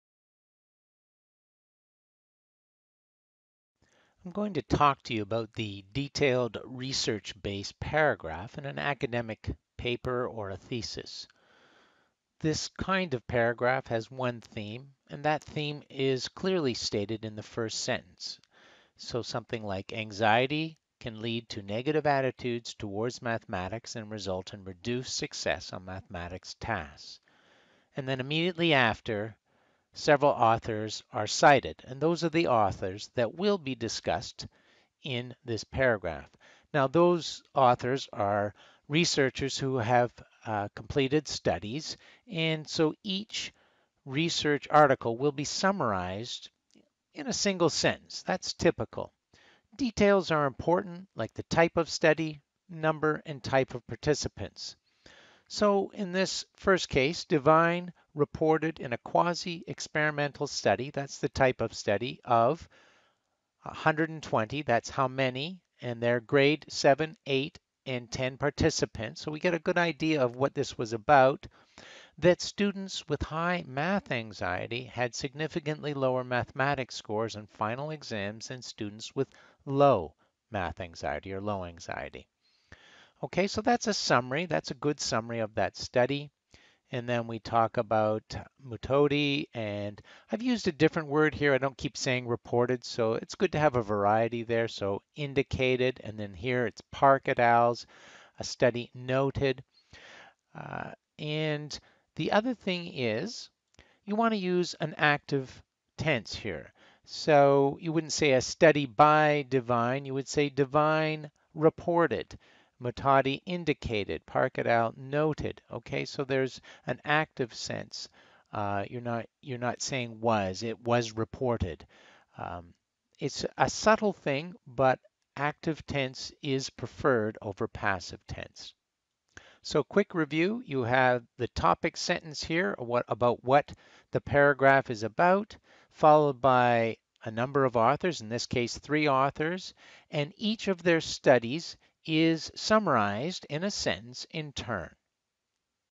Academic Writing - Research Paragraph
Privde a short explanation of how to write a reserach-based paragraph for an academice paper or thesis